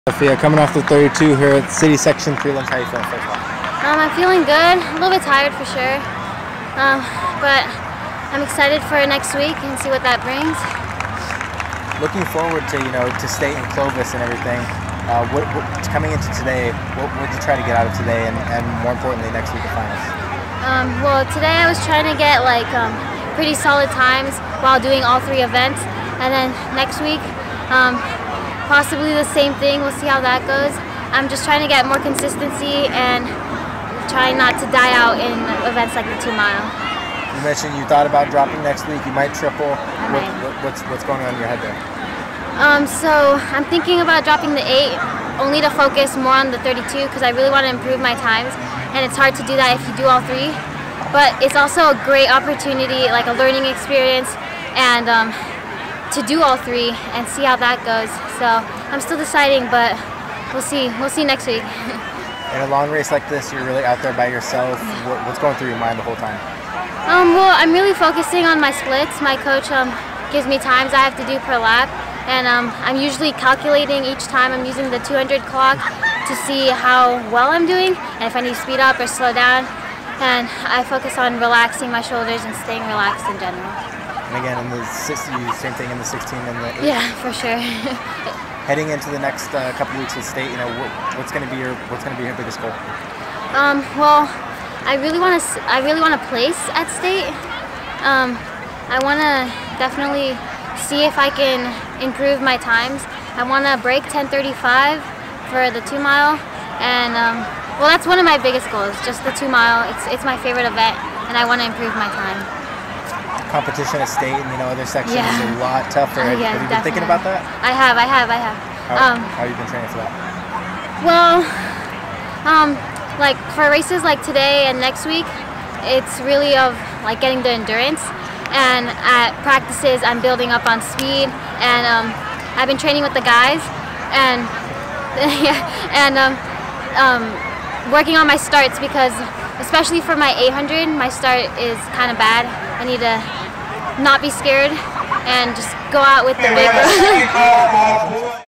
Granada Hills Charter's Sofia Abrego
Granada Hills Charter super freshman Sofia Abrego won her 800-, 1600- and 3200-meterraces at the 2019 L.A. City Section Prelims as she gears toward the section finals and the state meet. Here's her interview with PrepCalTrack's Dylan Stewart.